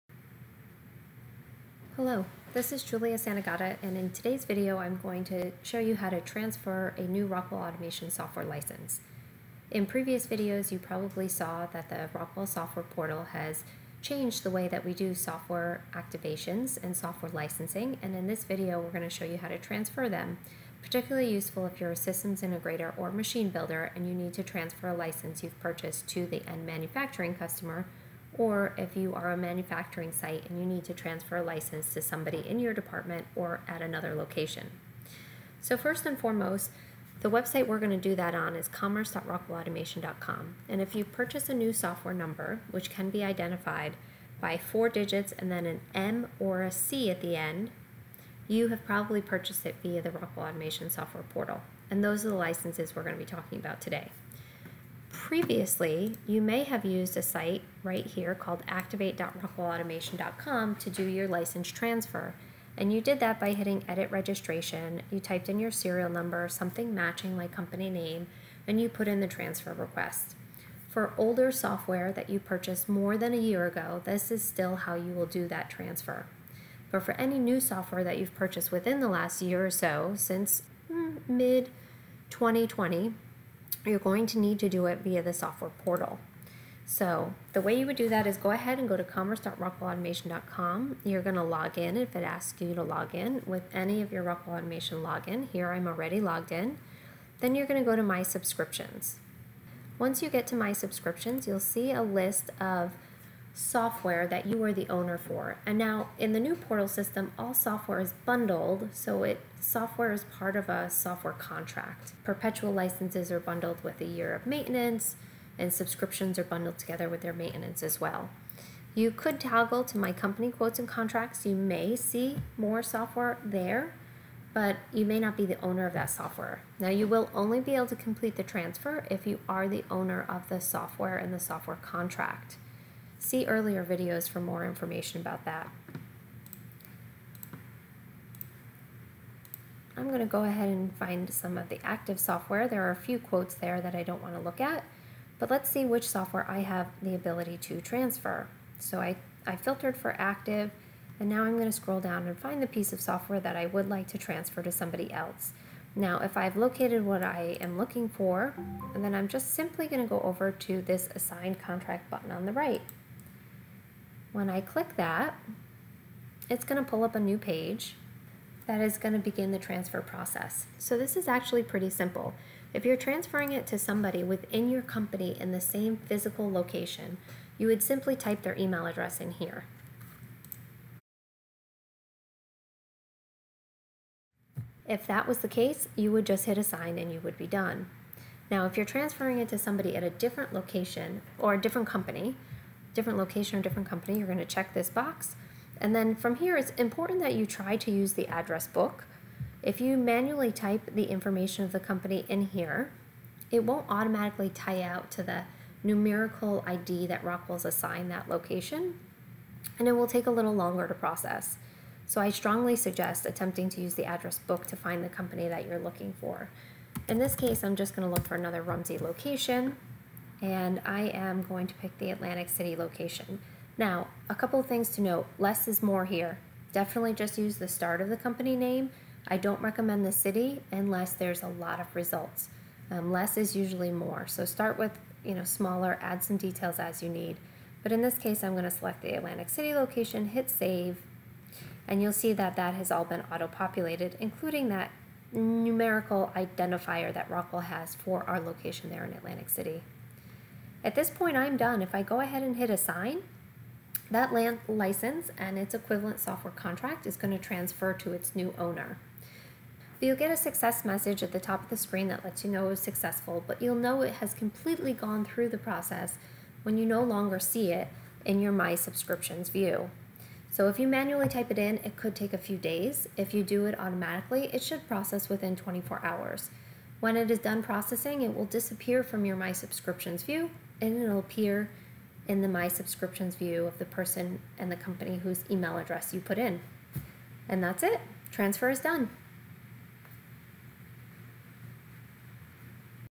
Rockwell Software Activations: How to Transfer a Software License
Learn the new way to transfer Rockwell Automation software licenses using the software portal.